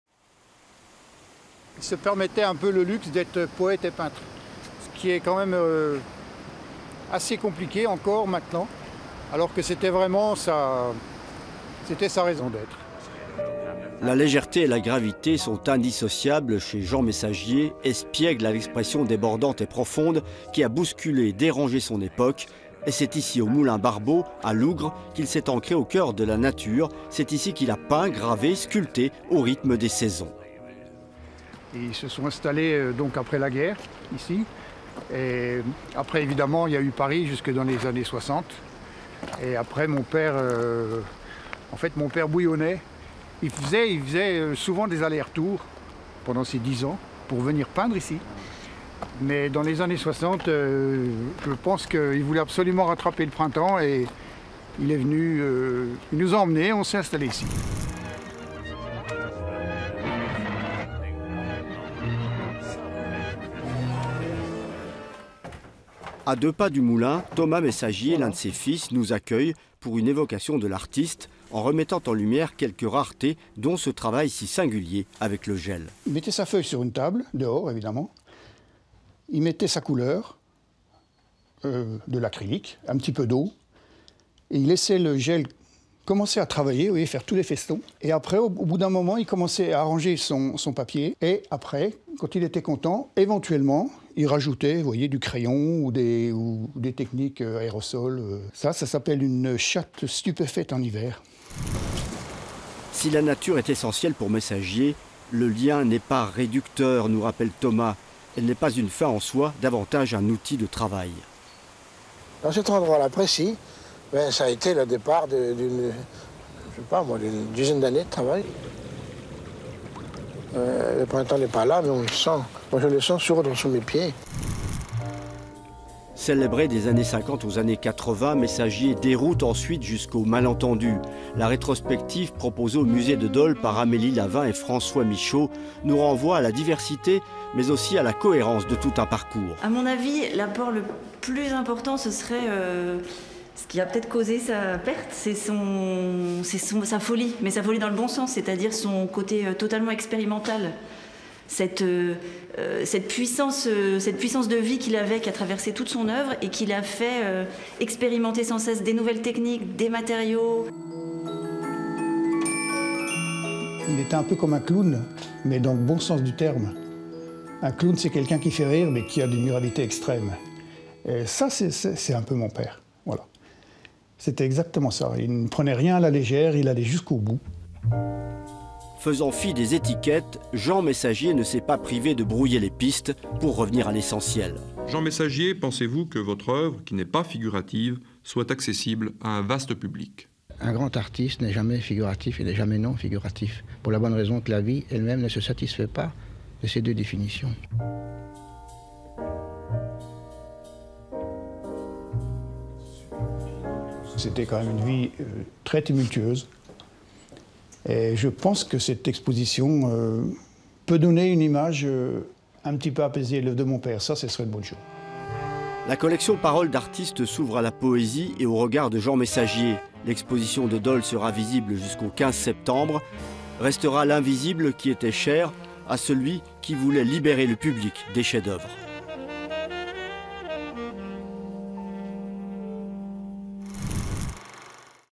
Mémoire en stock : Le printemps de Jean Messagier
A l'occasion de la grande rétrospective , " Tous les sexes du printemps", consacrée à Jean Messagier au musée de Dole, la rubrique Mémoire en stock revient sur l'évocation de cet artiste majeur, peintre, sculpteur, poète qui a passé l'essentiel de sa vie à Lougres près de Colombier-Fontaine dans le doubs.

Reportage M.Buzon, L.Brocard, F.Petit
avec
Thomas Messagier
Fils de Jean Messagier
Amélie Lavin
Directrice du Musée de Dole